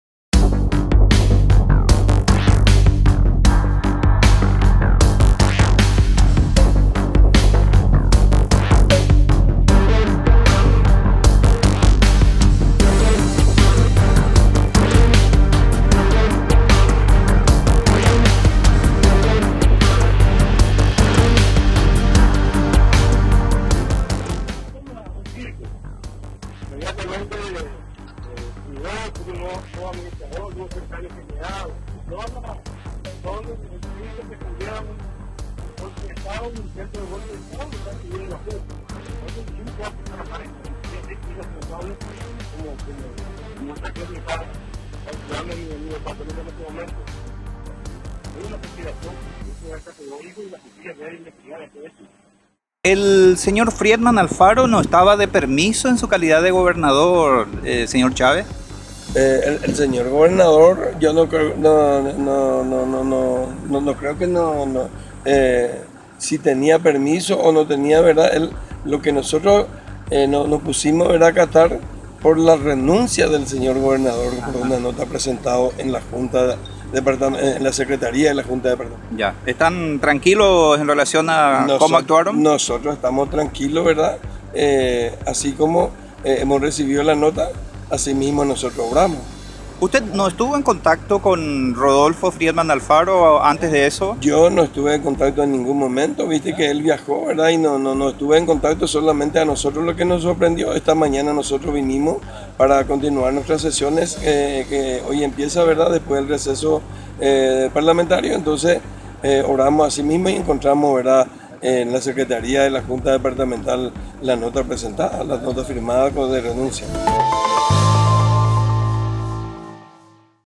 ¿Guairá tiene dos gobernadores?
En un día, dos gobernadores se disputan Guairá. Denuncias de falsificación y conspiración.